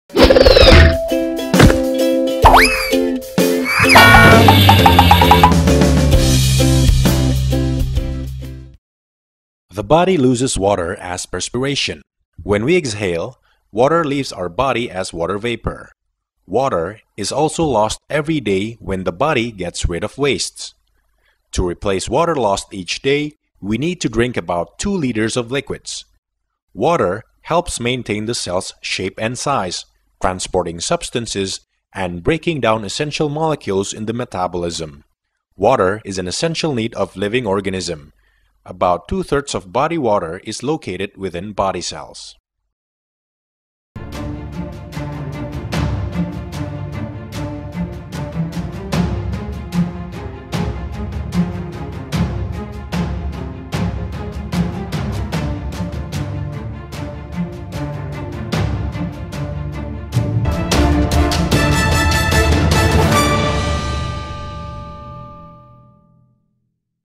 Why do You Feel Thirsty Learn for Children and Kids | EDUKID Laerning
When the body is dehydrated, the messages will be sent to the brain so it will result in feeling thirsty. Drinking water will dismiss the feeling of thirst and restore the body homeostasis.

To replace water loss every day, we need to drink about 2 L of water. Characteristics of healthy water are:  colorless, odorless, flavorless, and free of microorganisms contamination.